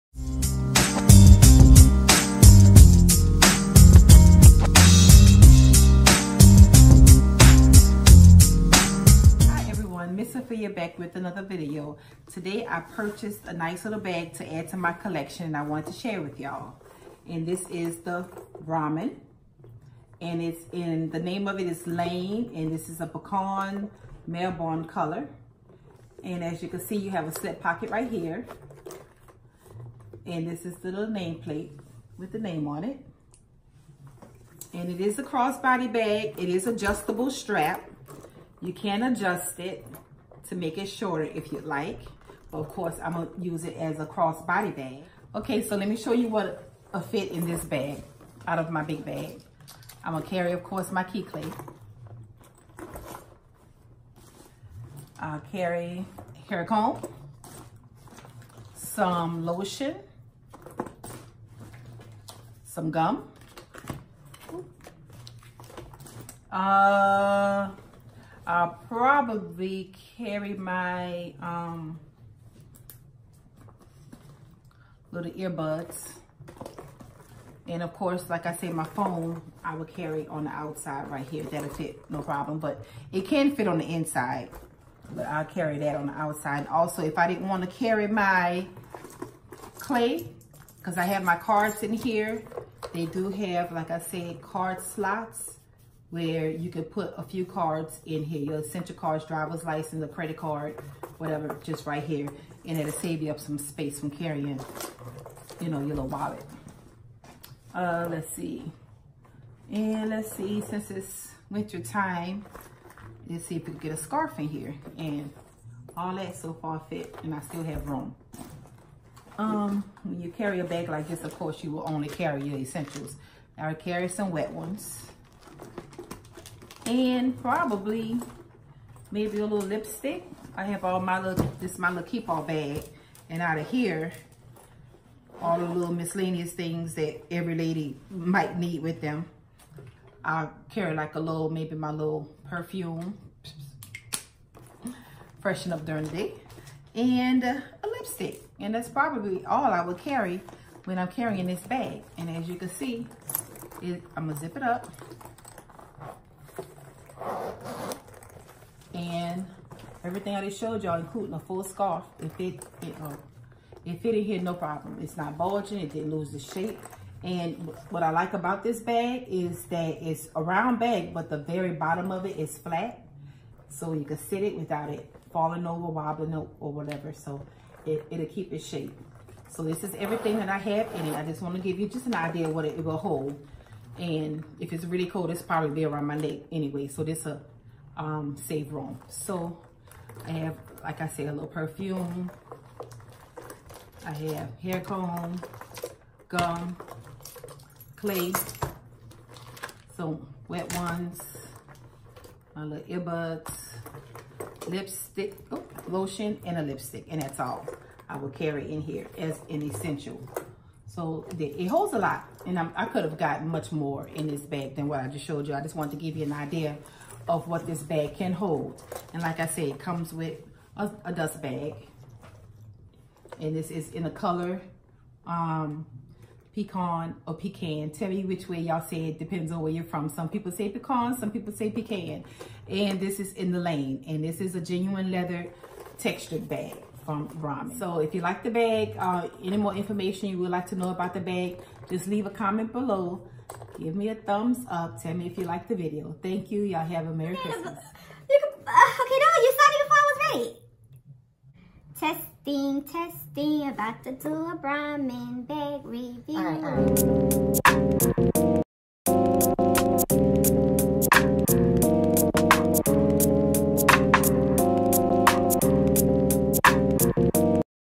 Brahmin-Lane review
Ms.Sophia’s review of brahmin lane crossbody bag!

I will be uploading more videos throughout the Christmas season!